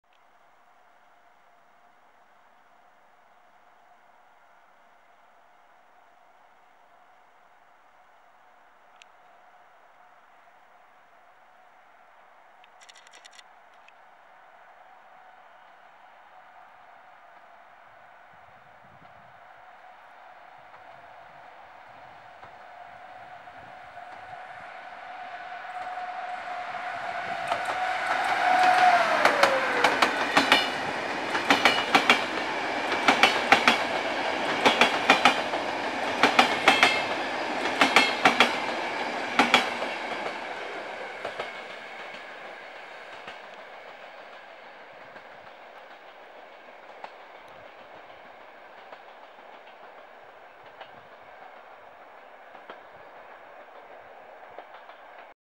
HUSARZ Z WAWELEM PO TORACH TOWAROWYCH
Nietypowo w dniu nagrania (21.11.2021) pociąg IC/EC 57 WAWEL relacji Berlin - Kraków prowadzony lokomotywą Husarz EU44-010 pokonywał odcinek Wrocław Główny - Święta Katarzyna po torach towarowych przez stację Wrocław Brochów. Tory towarowe są torami klasycznymi, a nie jak główne zasadnicze bezstykowymi stąd charakterystyczny stukot kół.